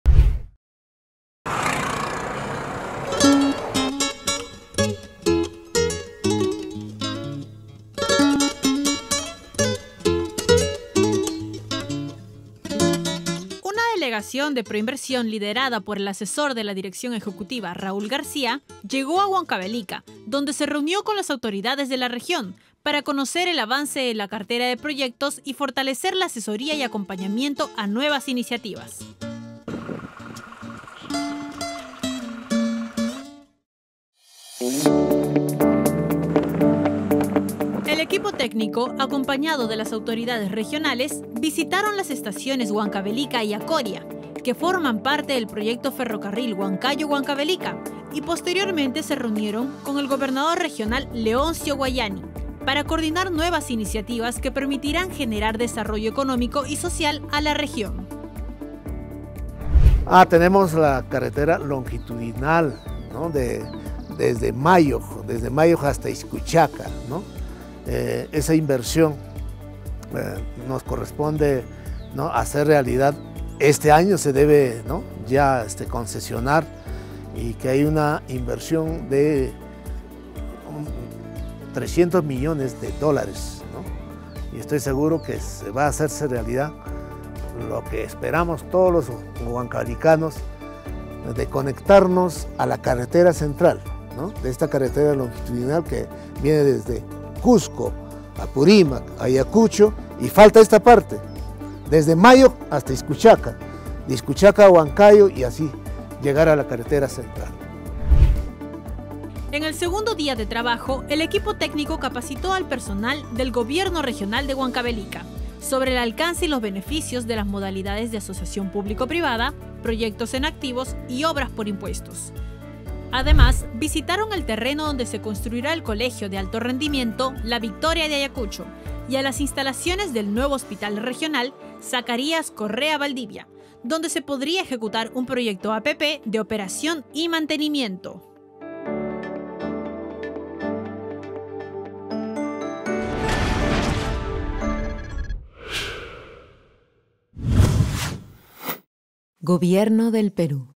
ProInversión en regiones: Huancavelica
Desde Huancavelica, el equipo técnico de PROINVERSIÓN, encabezado por el asesor técnico de la Dirección Ejecutiva, Raúl García, sostuvo reuniones con autoridades de la región para impulsar y fortalecer la cartera de proyectos en la región, como el Ferrocarril Huancayo-Huancavelica y la Carretera Longitudinal de la Sierra Tramo 4.